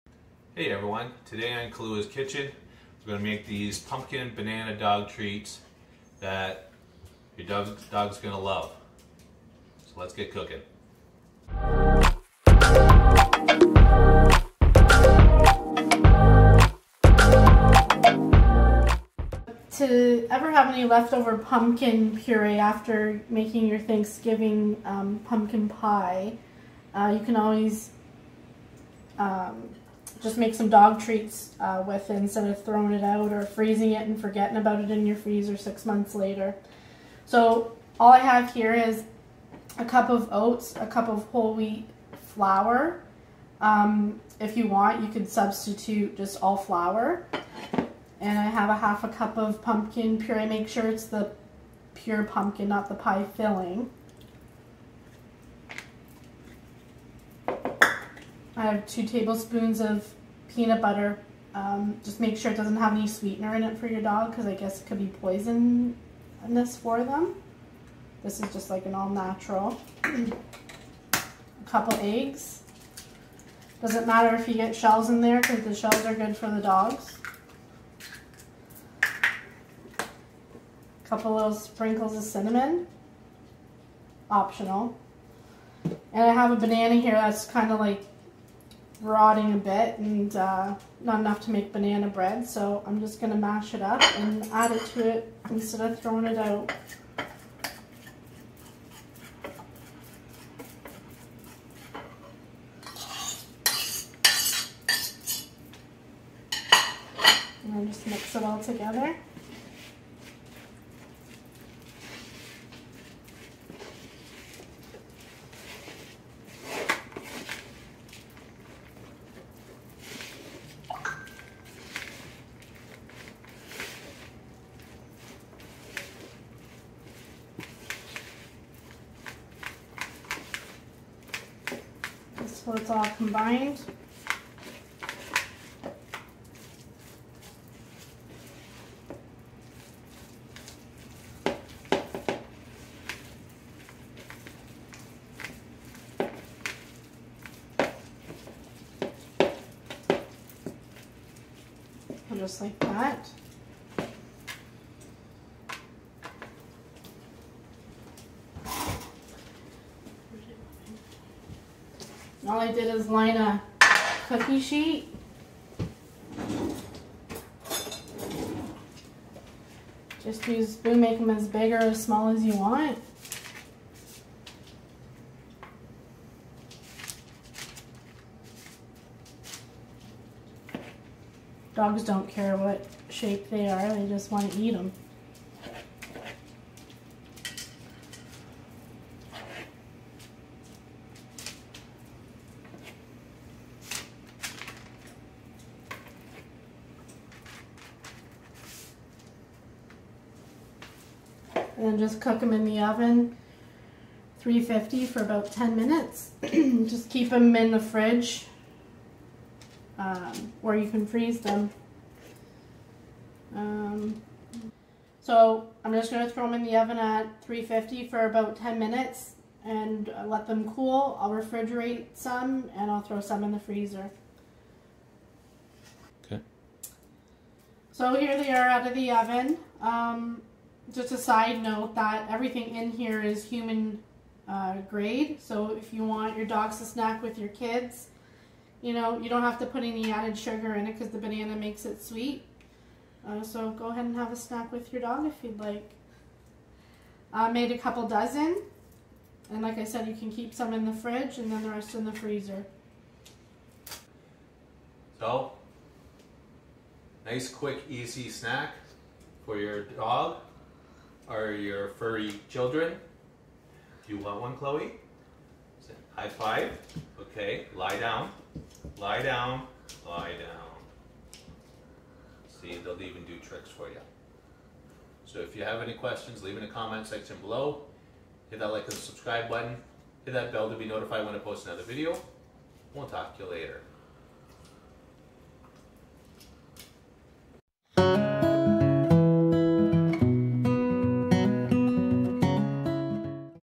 QUICK, EASY, HEALTHY PUMPKIN DOG TREATS FOR YOUR FUR BABIES!!! THEY WILL LOVE THEM!!!
RECIPE:

1 cup oats
1 cup whole wheat flour
1/2 cup pumpkin puree
2 tbsp all natural peanut butter
2 eggs
1 ripe banana
1/4 tsp cinnamon

Bake in the oven at 350 F for 10 minutes